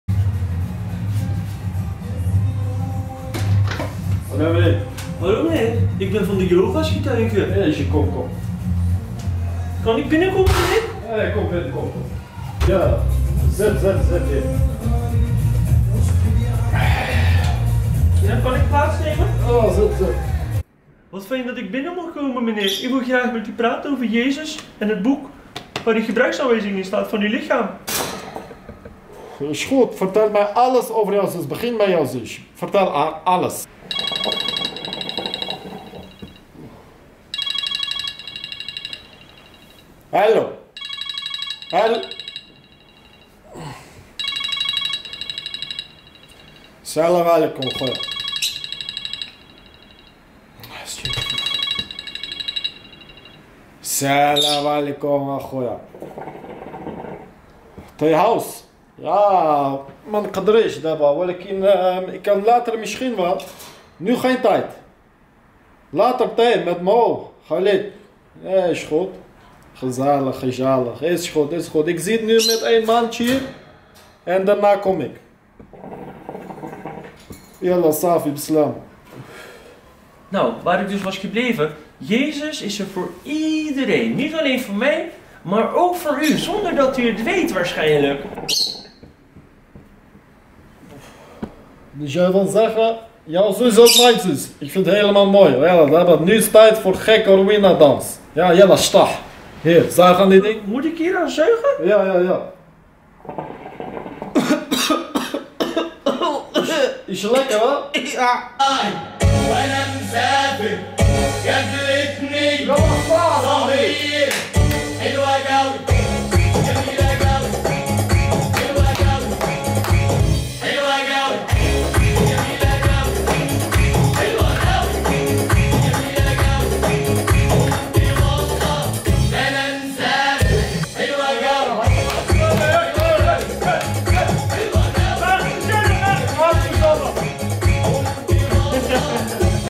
Wat gebeurd er als een jehova getuige aan de deur komt van een moslim.?
Jehova dwaalt af van zijn route